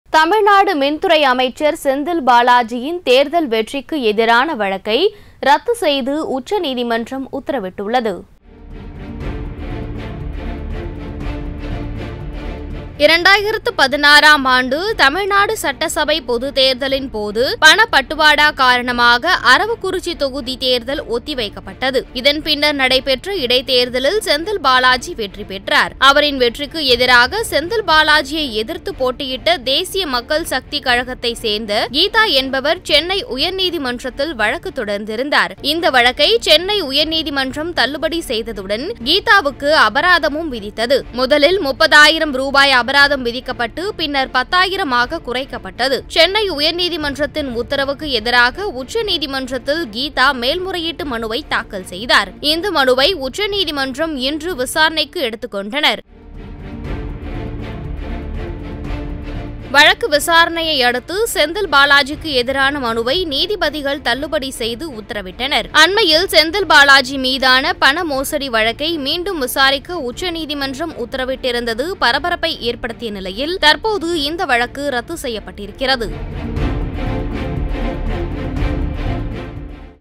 செந்தில் பாலாஜிக்கு எதிரான வழக்கில் உச்சநீதிமன்றம் அதிரடி தீர்ப்பு | Minister Senthil Balaji | DMK
செந்தில் பாலாஜிக்கு எதிரான வழக்கில் உச்சநீதிமன்றம் அதிரடி தீர்ப்பு